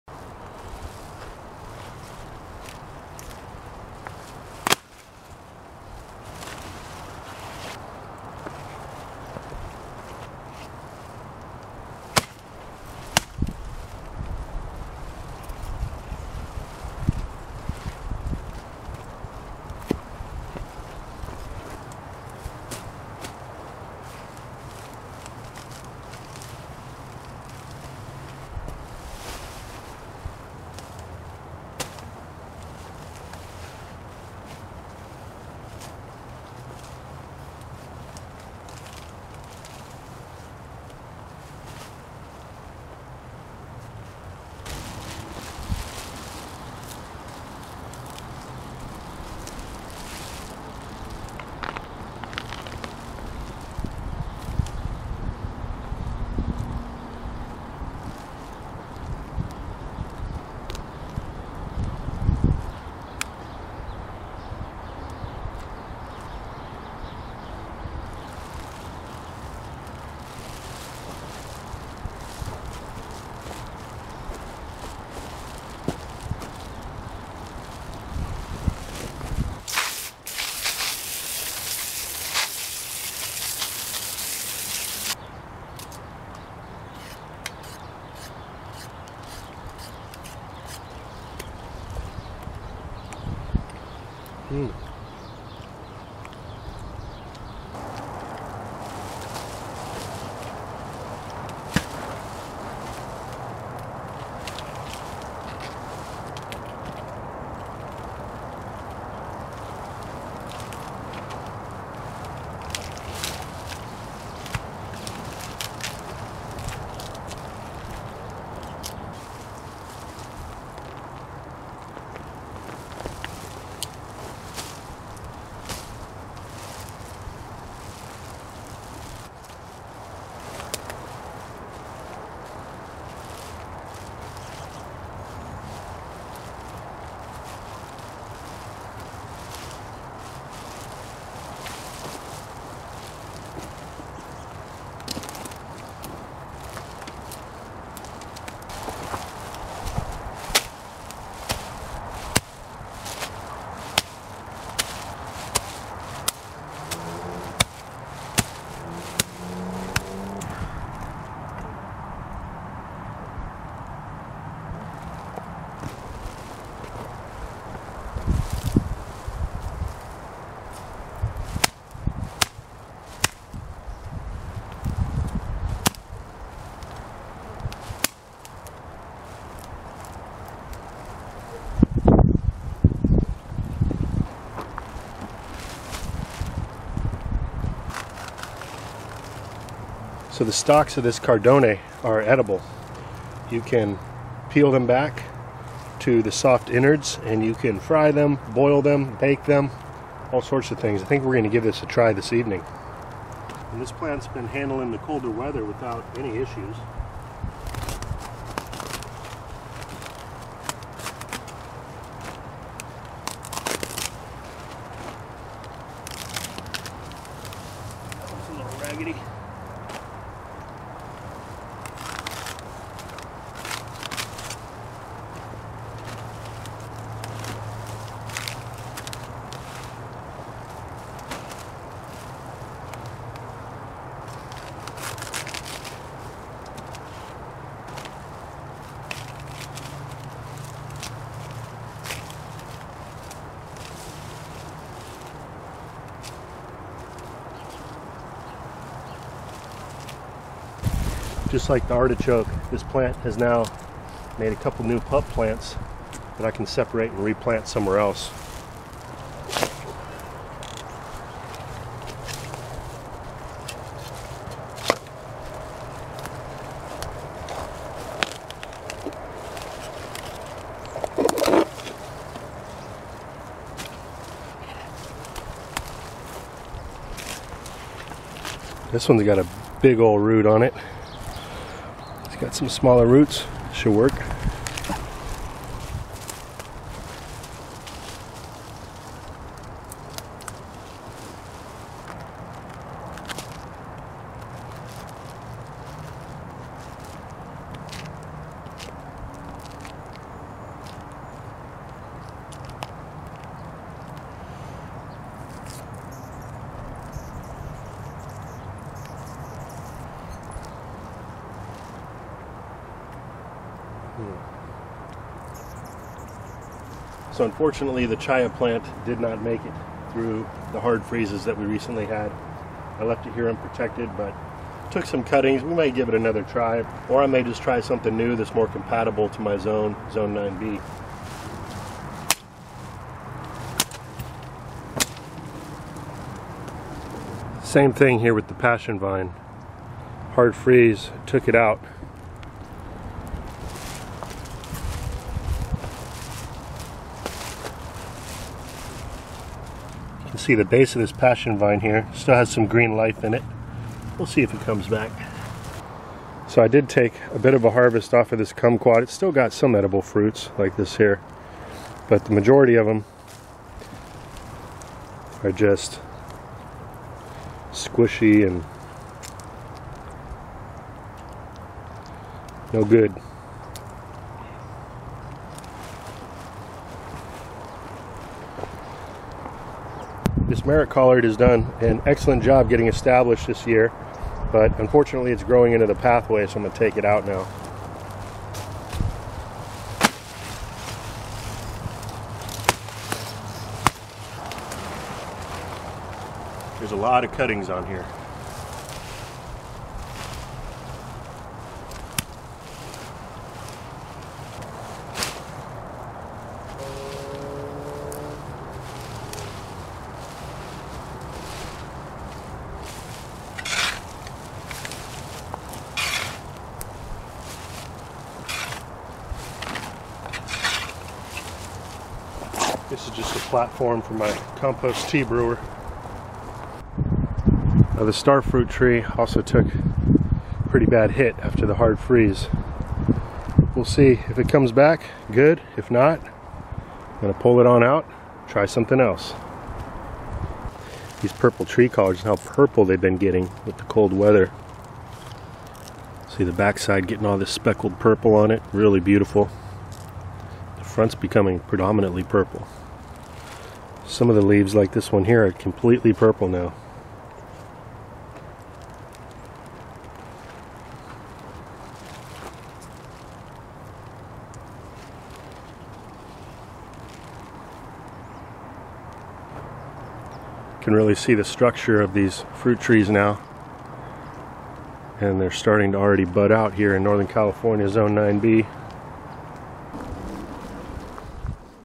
All in a Day's Work! Plus Results After The Hard Freeze!
takes you along as he goes about preforming some routine winter maintenance throughout the garden. After a few days of hard freezes here in Northern California zone 9b some of the experimental plants unfortunately didn't make it.